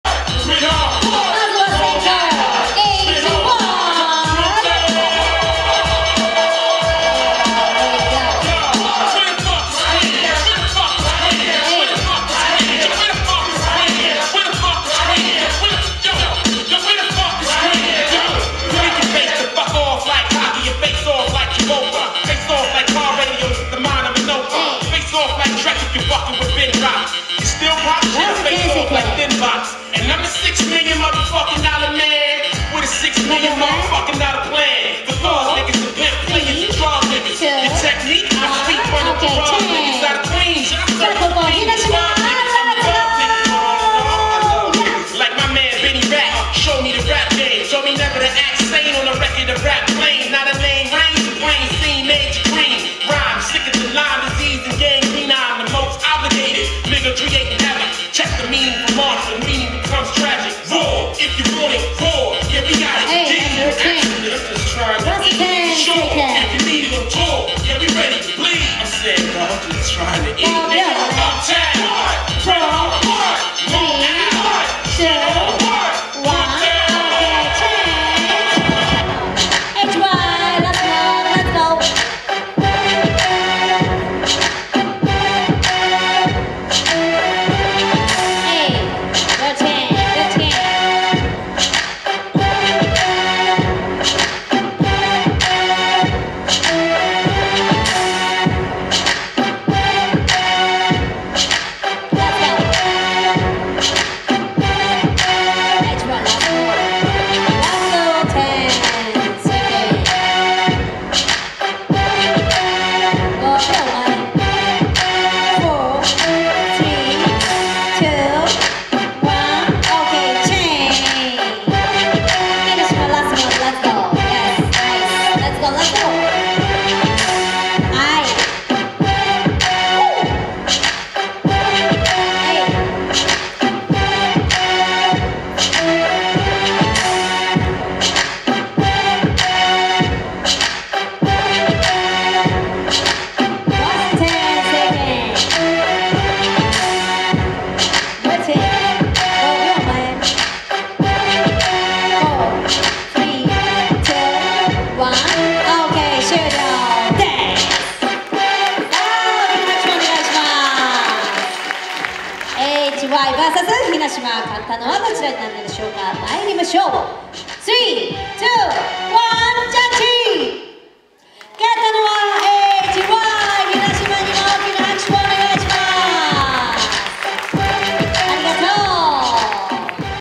【D’YNAMICダンスフェスティバル】【ひなしま vs H・Y】
2023年9月18日に、調布市文化会館たづくりーくすのきホールにて開催された【D’YNAMICダンスフェスティバル】でのダンスバトル（2on2 ）の１回戦【ひなしま vs H・Y】です。

H・Y